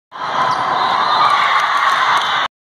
Club One 15 Gold Volleyball - Day 2 - SCVA
Video clip from Club One 15 Gold Volleyball tournament in Las Vegas